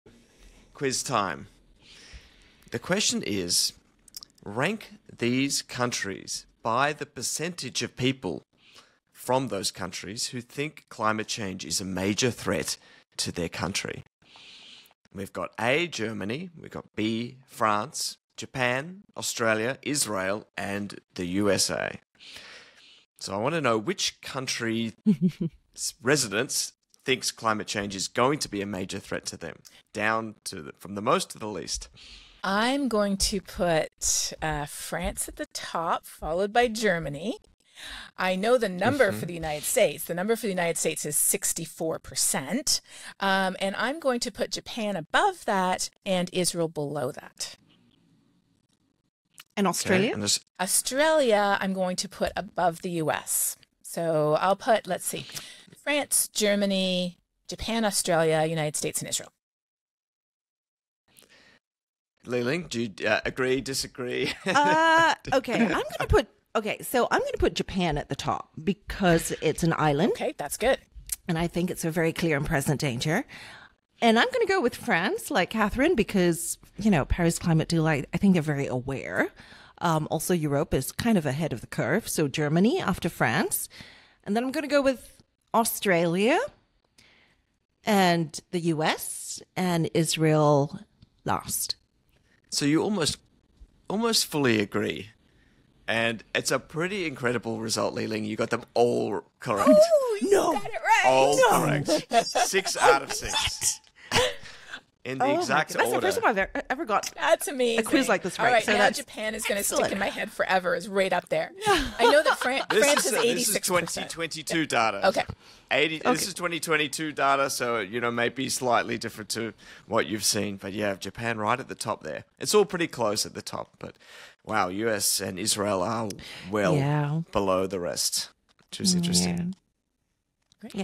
Which countries think climate change is a major threat? | Climate Conversations podcast
Rank these countries by the % of people who think climate change is a major threat. Did you get it correct?